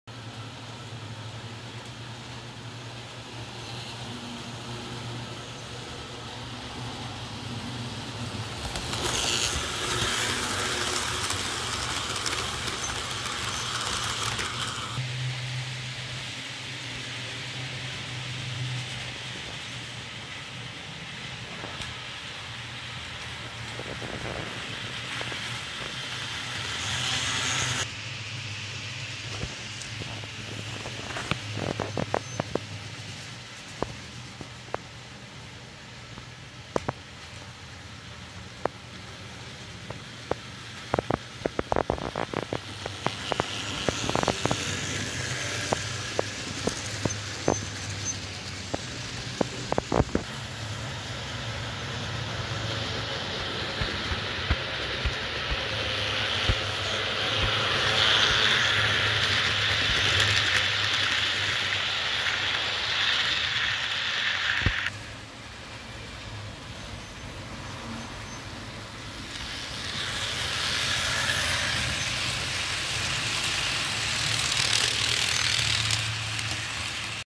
bnsf w/up run thru power pulling mixed freight #533&#9500
bnsf w/union pacific run thru power pulling mixed freight on my brothers layout.